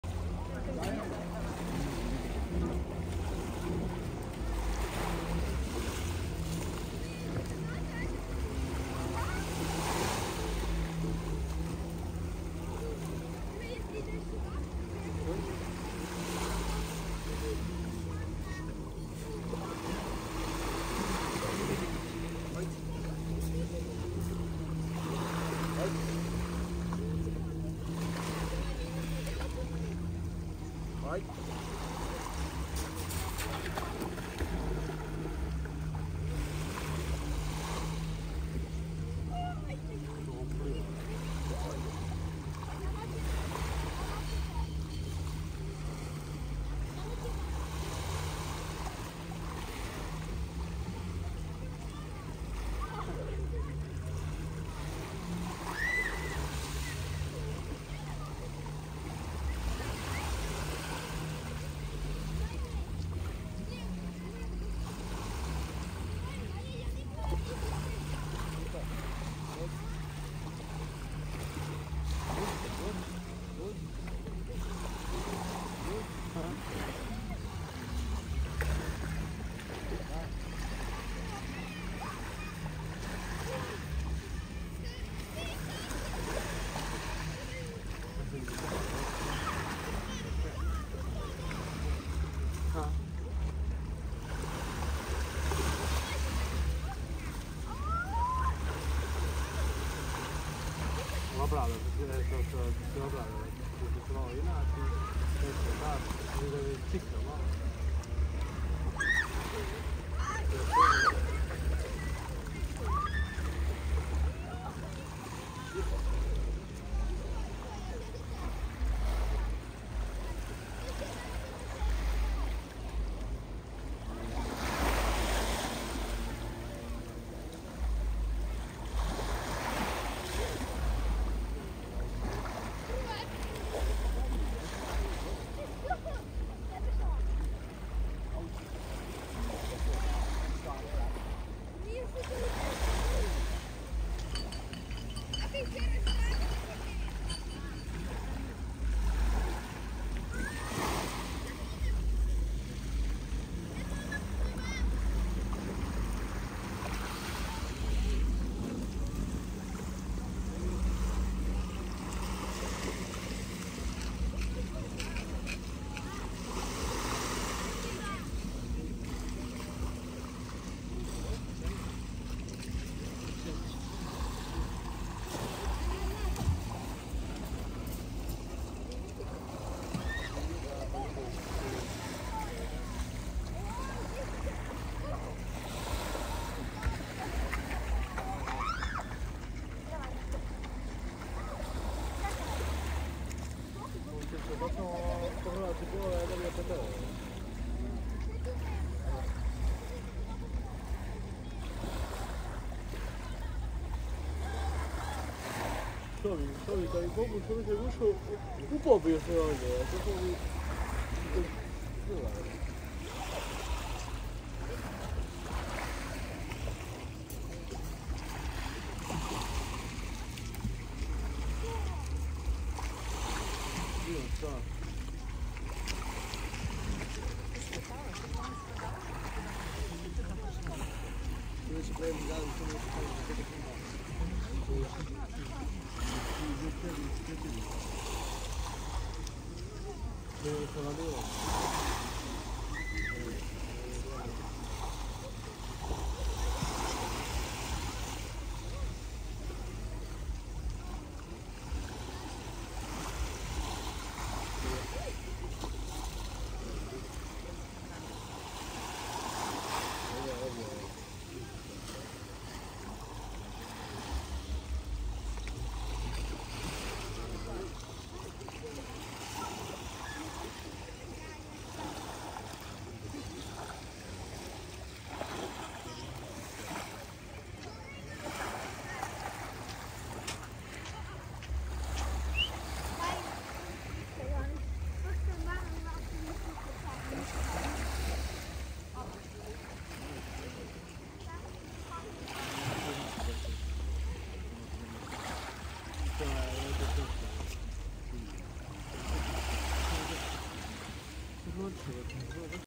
Evening beach near the walls of the old town of Budva
People are resting, a white labrador came ashore, who also wants to have a great time and swim in warm water